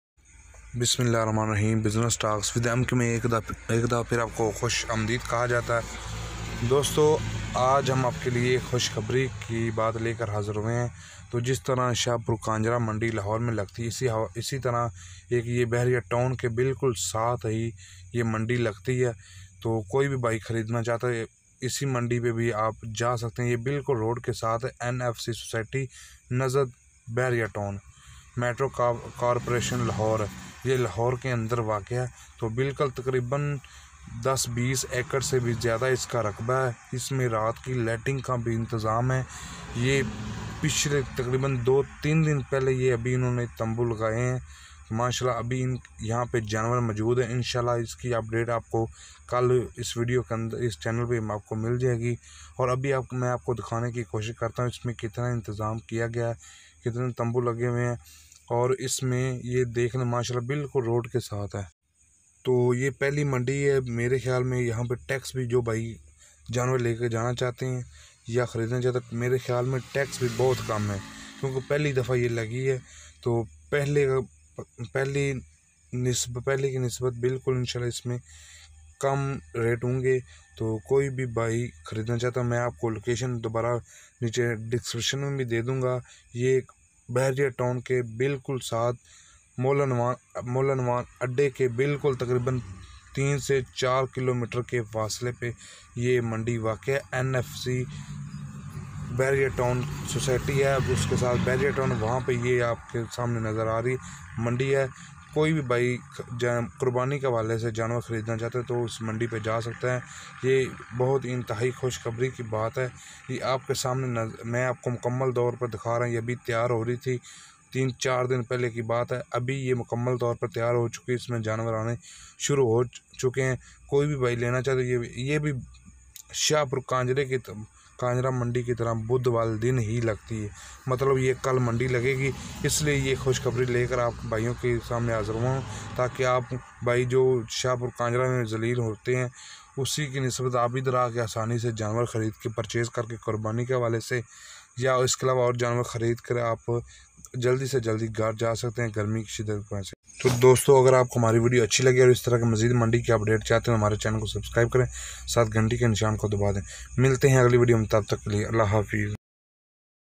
Good news! Another huge market was set up in Lahore-Buy cheap Vacha from this market for sacrifice
خوشخبری! لاہور میں ایک اور بہت بڑی منڈی لگ گئی | وچھا منڈی-اس منڈی سے  قربانی کیلئے سستے وچھے خریدیں